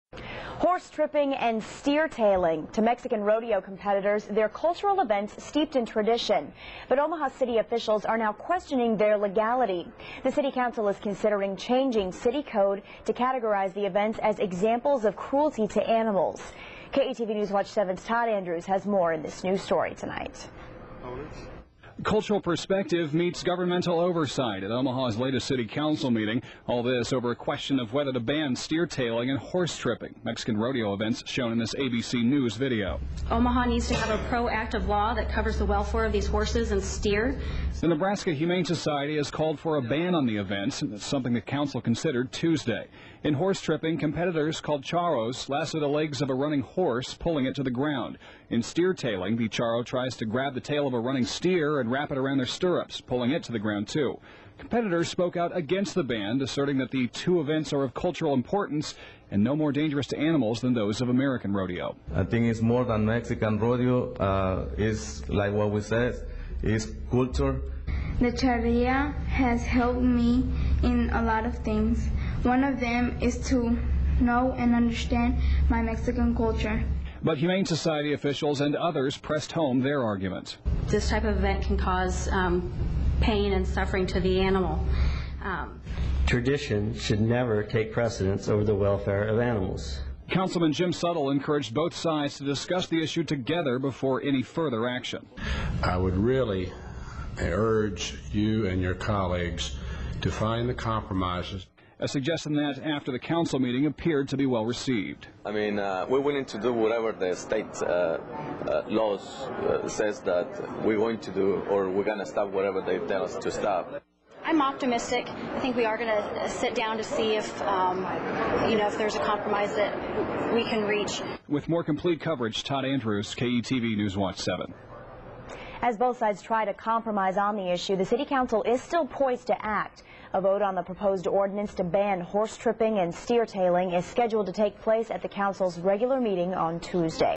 Omaha Considers Banning Mexican Rodeos
Some Mexican rodeo traditions are in question in Nebraska.